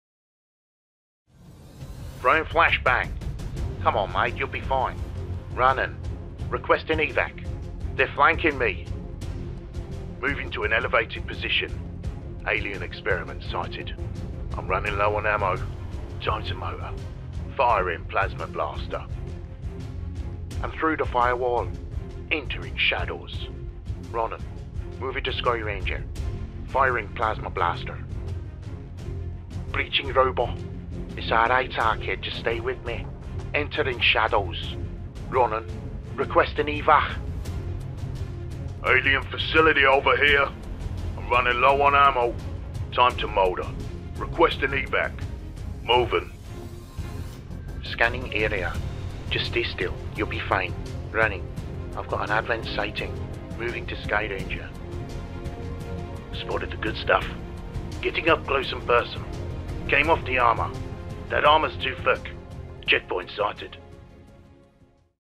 [LWOTC} Xcom 2 Long War of the Chosen Soldier voices
Hi all. I know I am very late to the party but I have a ton of hours sunk into XCom and LW2 already and I thought it is time to give back to that community and try to progress my va career at the same time as it makes for great practice.

However, I don't have access the sdk of Xcom 2 so I can't put a mod together with these voices and that is where I am reaching out to the community to see if anyone has similar interests concerning mods for LWOTC.

As there are swear words in the content and mentions of religious figures, I think that Firaxis wouldn't be that interested in it. But here's hoping.

You can find the 500+ soundbites in my googledrive folder and everyone is welcome to use them however but if someone could slap that mod together. Please get in touch.